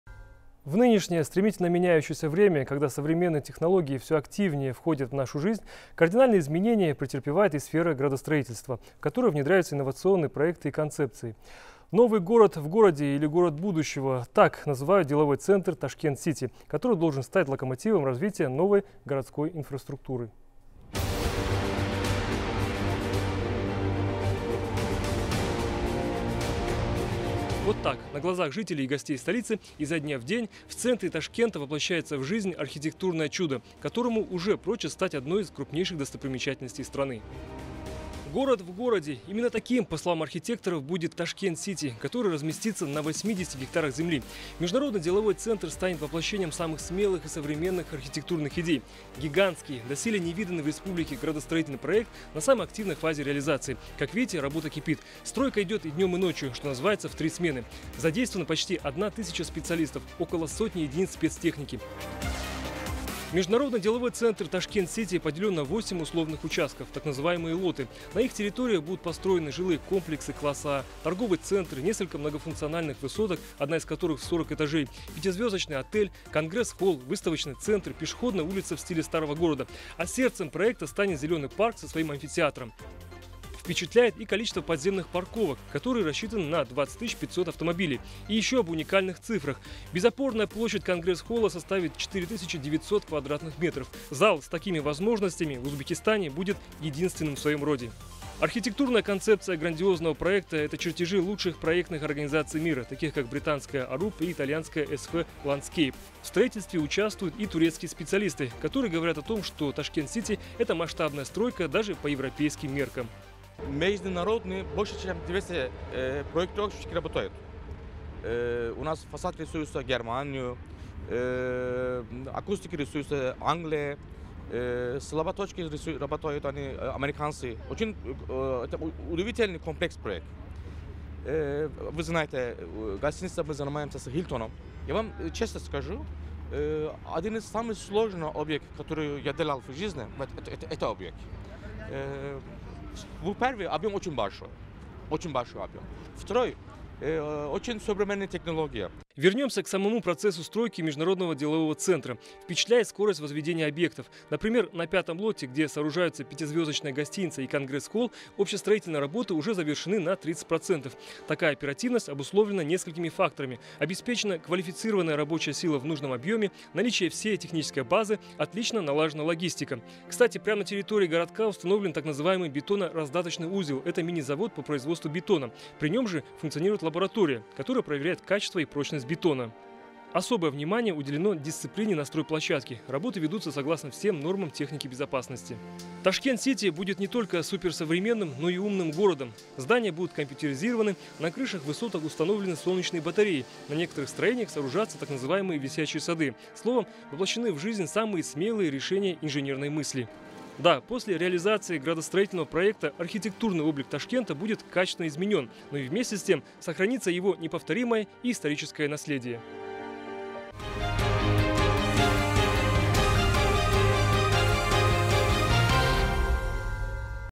Больше, чем cтройка
Город будущего – так называют масштабный строительный проект МДЦ Tashkent City. Репортаж на канале Uzbekiston 24 рассказывает про архитектурное чудо - самое масштабное строительство в Узбекистане.